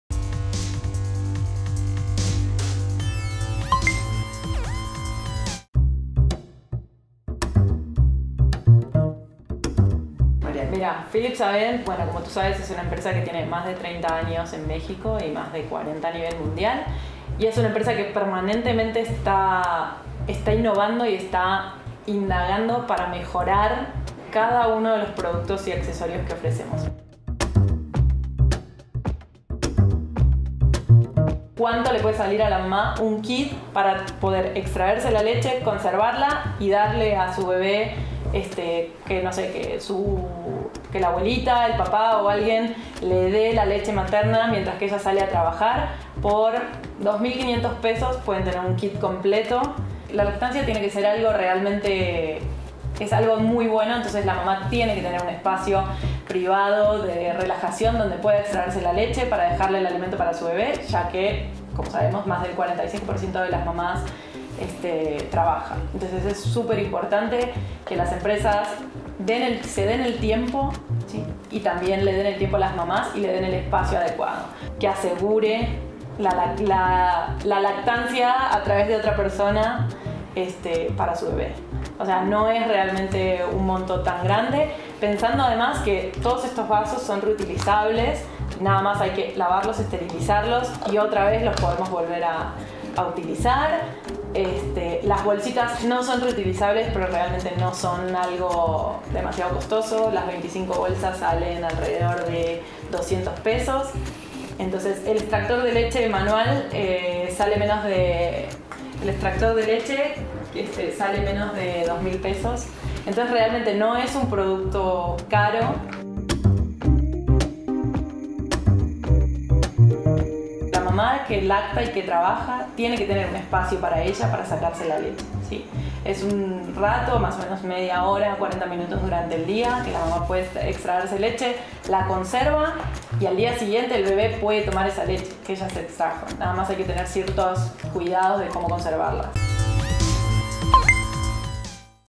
Entrevista a Mariana Paván de Philips Avent México
My Press entrevistó a Mariana Paván, Sr. Marketing Manager Mother Child Care de Philips Avent quien cuenta sobre los avances de Philips en el área de productos y accesorios para lactancia.